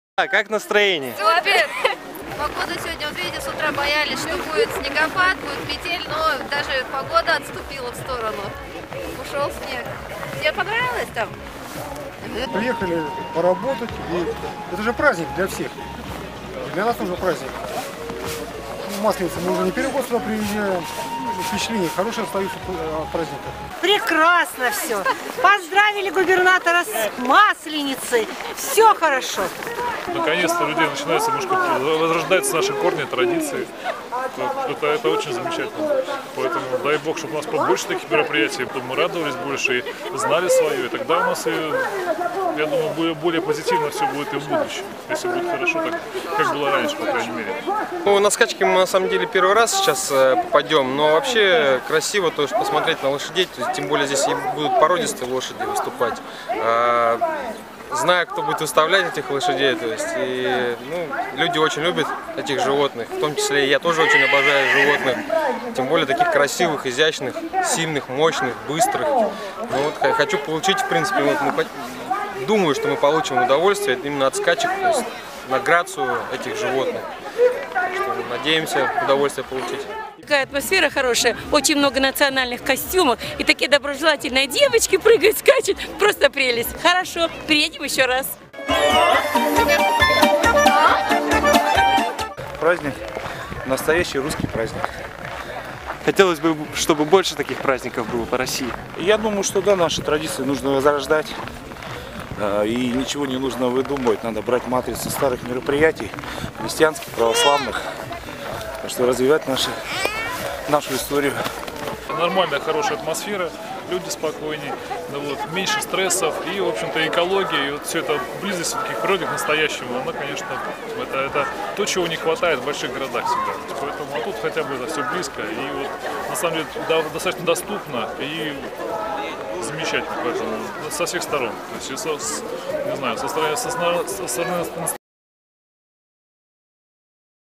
Сибирская Масленица 2014 - Люди говорят
Сибирская Масленица 2014 - Сибирское подворье, село Новотырышкино Алтайског края